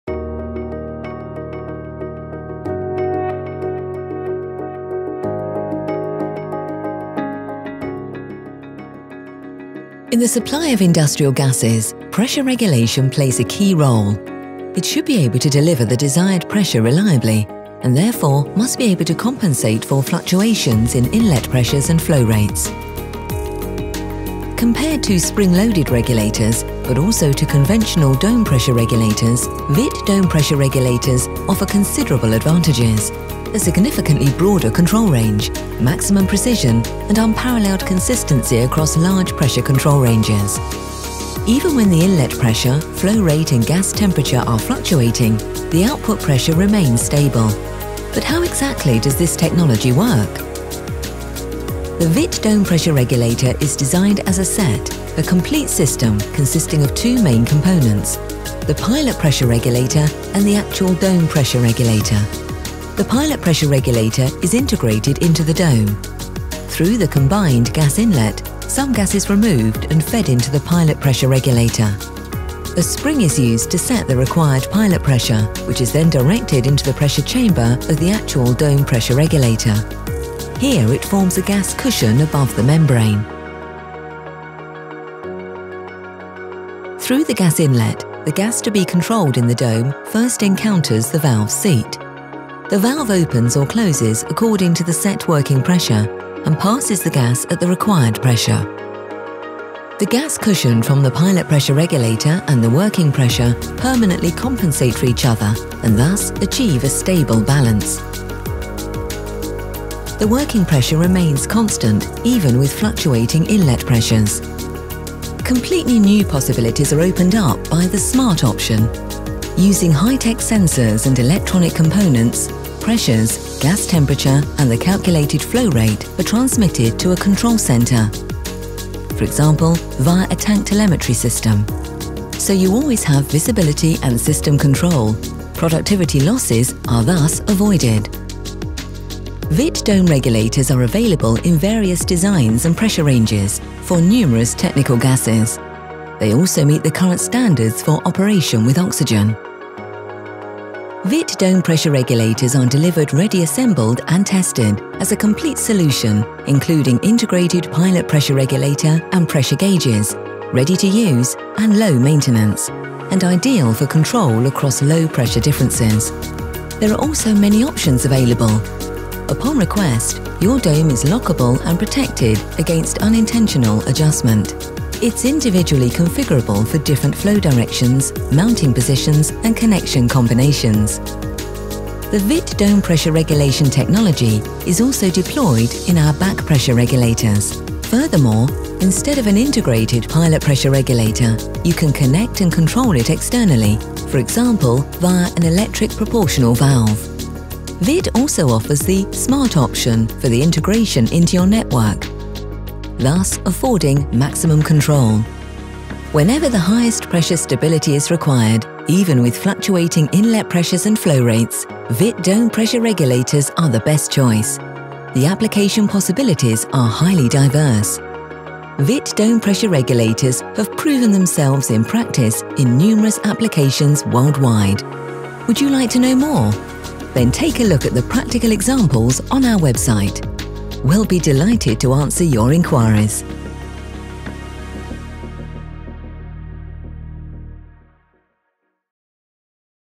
Dome-loaded gas pressure regulator valves - 3D animation - function and benefits
Compared to spring-loaded gas pressure regulators or reducers and to conventional dome pressure regulators, WITT dome-loaded pressure regulators offer considerable advantages:
- a significantly broader control range
- maximum precision
- unparalleled gas pressure consistency across large pressure control ranges.
Even when the inlet gas pressure, flow rate and gas temperature are fluctuating, the outlet gas pressure remains stable.

Other names are fluid-loaded or pilot-controlled pressure regulators or pressure reducing valves.

How does a dome-loaded pressure regulator work?

The WITT Dome-loaded Pressure Regulator is  a complete system consisting of two components: the pilot gas pressure regulator and the actual dome pressure regulator.
The pilot pressure regulator is integrated into the dome. Through the combined gas inlet, some gas is removed and fed into the pilot pressure regulator. A spring is used to set the required pilot pressure, which is then directed into the pressure chamber of the actual dome pressure regulator. Here it forms a gas cushion above the membrane.
Through the gas inlet, the gas to be controlled in the dome first encounters the valve seat. The valve opens or closes according to the set working pressure and passes the gas at the required pressure. The gas cushion from the pilot pressure regulator and the working pressure permanently compensate for each other and thus achieve a stable balance. The working pressure remains constant, even with fluctuating inlet pressures.